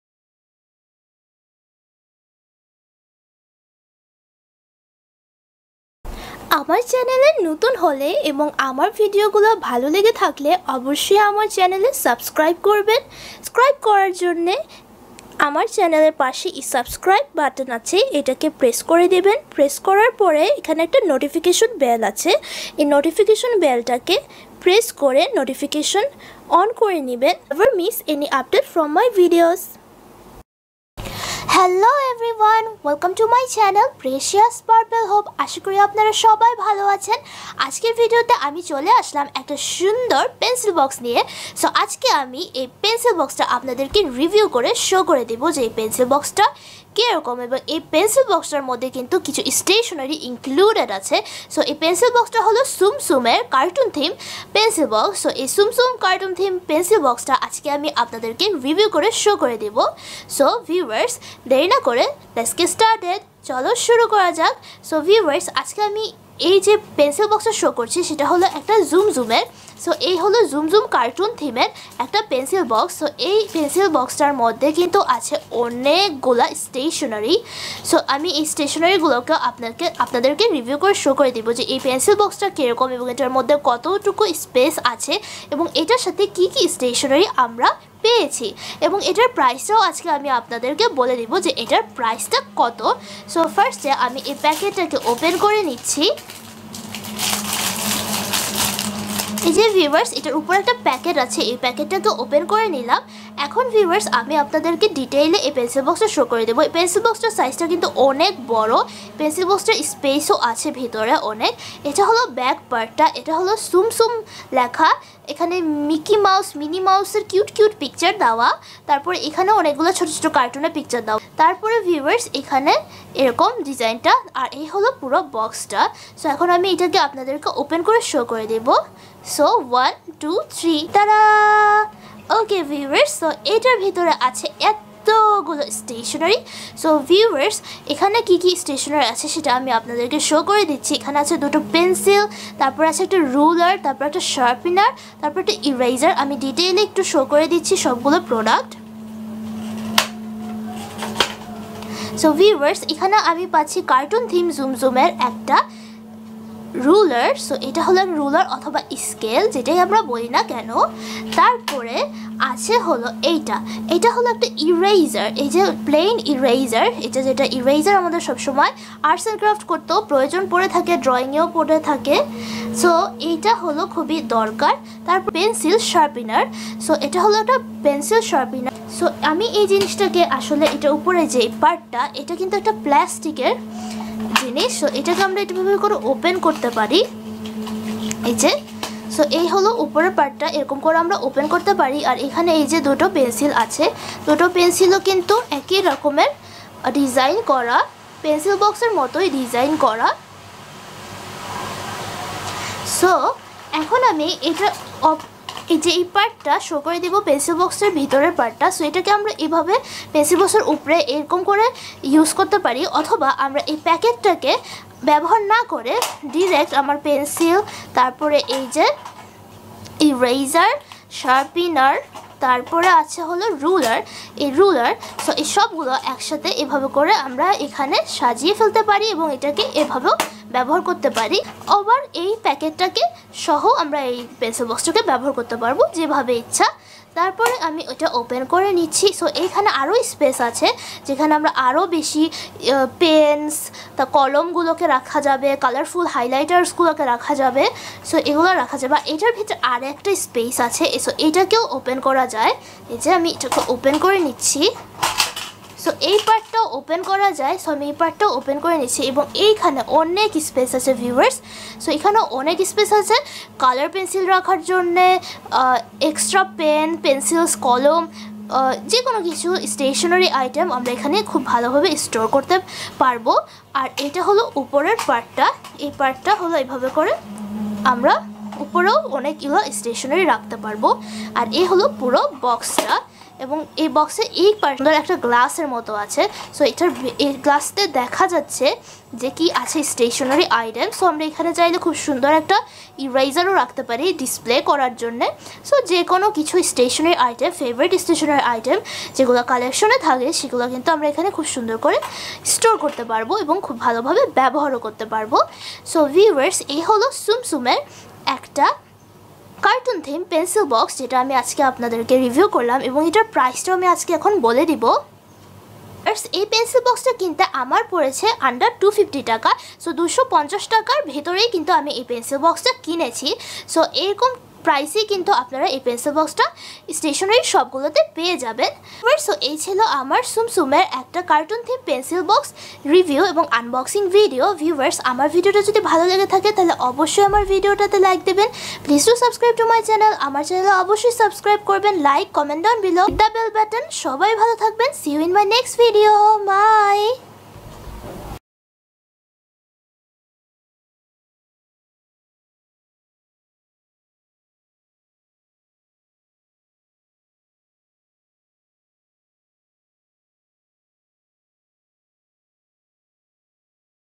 Tsum Tsum cartoon theme pencil box with stationery review for kid's.
Precious purple hope...
In this video..i am going to show you...these awesome tsum tsum cartoon theme pencil box with stationey review for kid's...
this cartoon theme pencil box is really good for kid's...it has a lot of space inside to store more essential stationery..i rally like this pencil box...it also comes with a medium size rular..one eraser...one pencil sharpener and two pencil...
This is not a sponsored video...this is what i use and like most..and like to share with you viewers...
Thanks a lot for watching it...
see you in my next video...
.Bye.